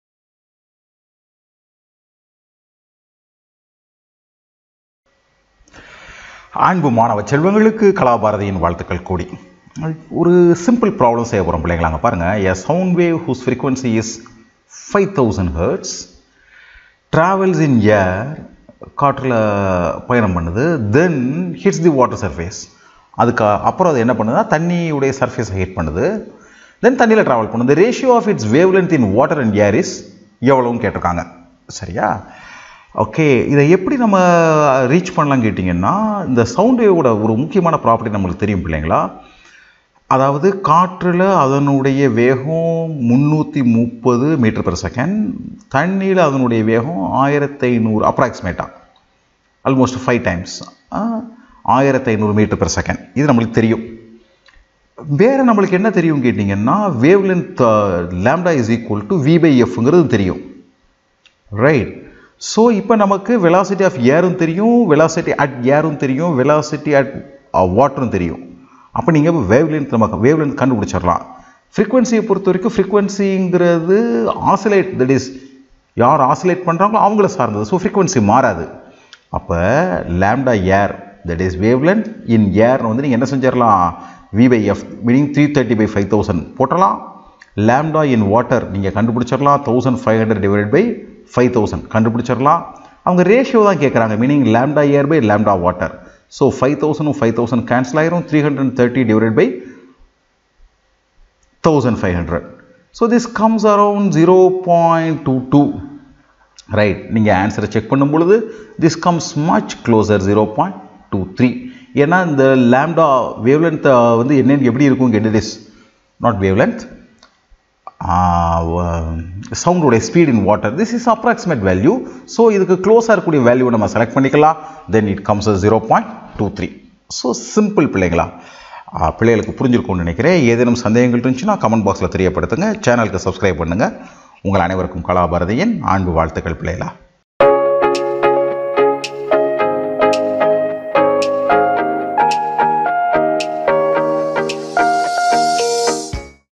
37.Physics | Waves | Multiple Choice question 6
A sound wave whose frequency is 5000 Hz travels in air and then hits the water surface. The ratio of its wavelengths in water and air is